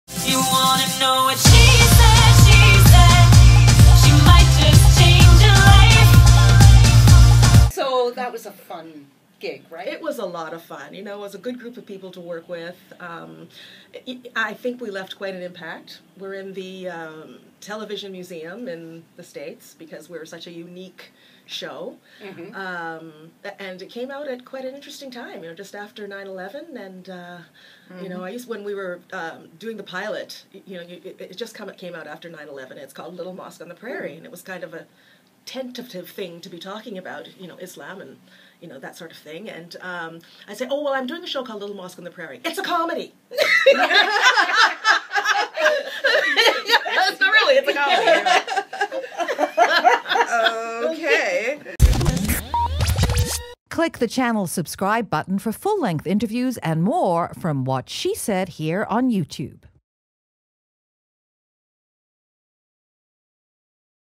remembers 'Little Mosque on the Prairie'
"It's a comedy!" Award-winning singer, actor, writer, artist and entrepreneur Arlene Duncan​, best known for her six seasons on CBC's Little Mosque on the Prairie joined us on Tuesday and told us about her experience when she first started the show.

Listen to What She Said weekdays on SiriusXM Canada Talks channel 167 from 10ET-noon.